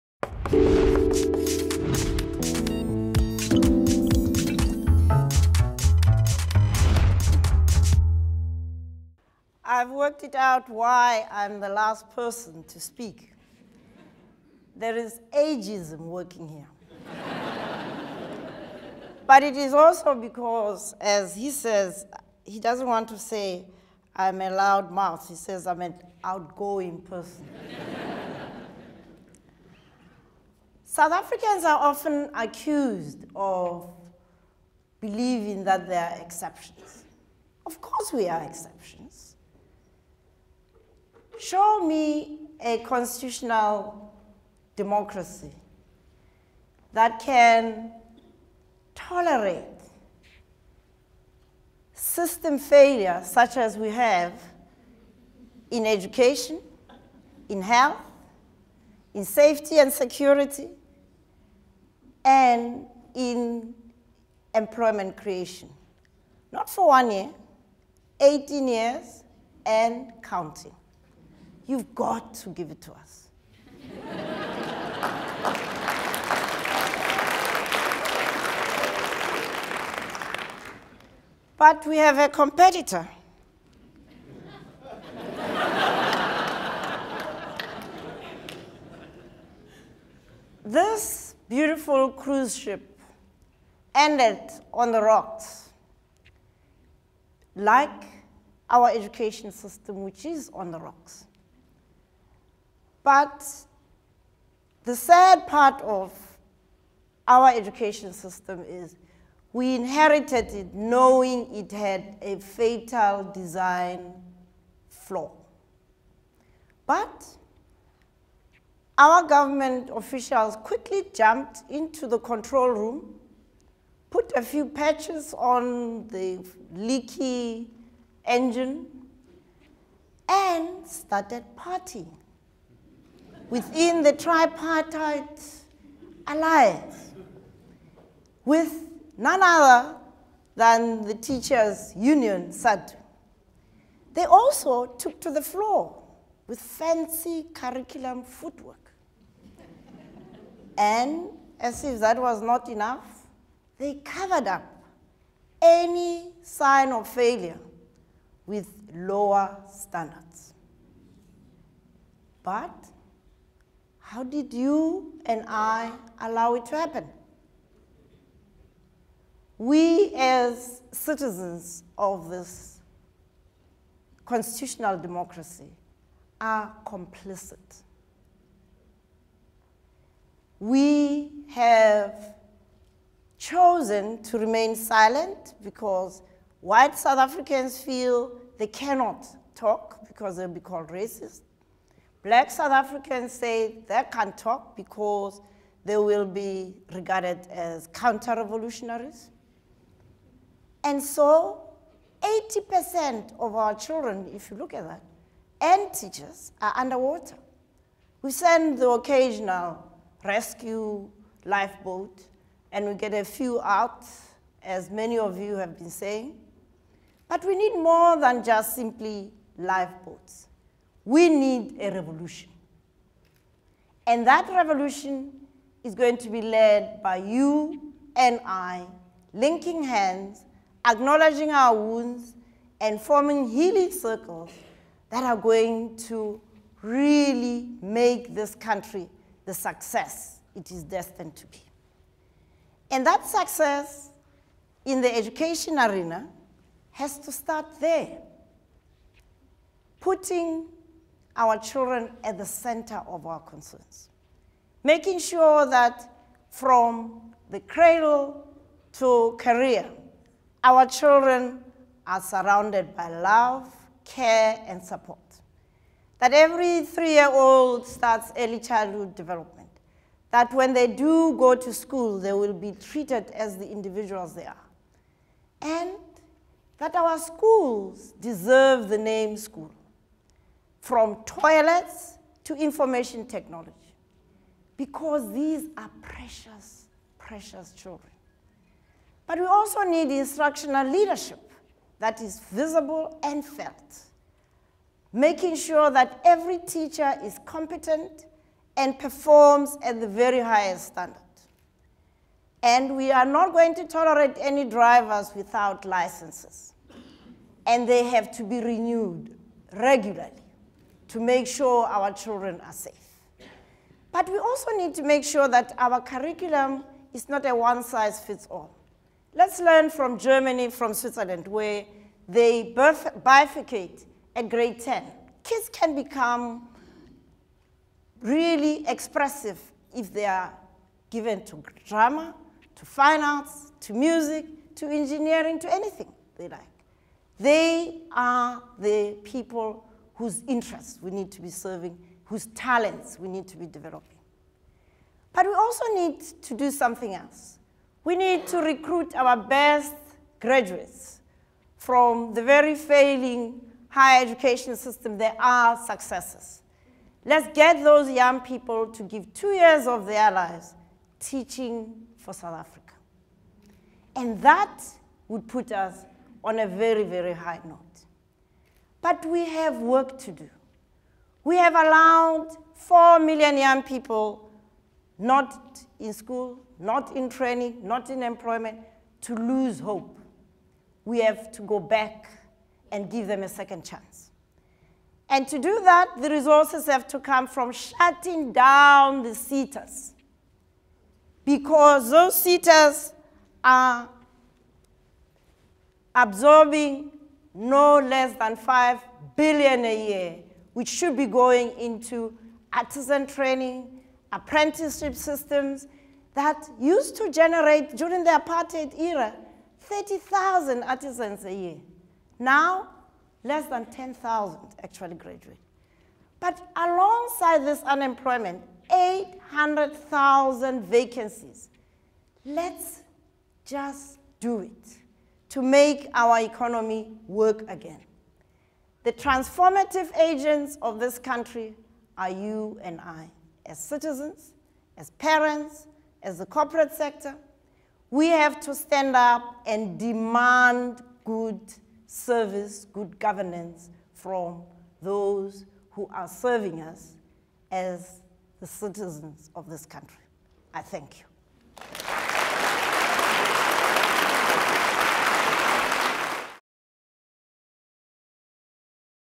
Rising to our citizens' responsibility: Dr Mamphela Ramphele at TEDxCapeTownED
Dr Ramphele is a South African academic, businesswoman and medical doctor and was an anti-apartheid activist. She was one of the founders of the Black Consciousness Movement.  In this short talk from TEDxCapeTownED, she issues a strong rebuke for the passivity of South African citizens and explores some changes that could profoundly improve the standard of education in South Africa.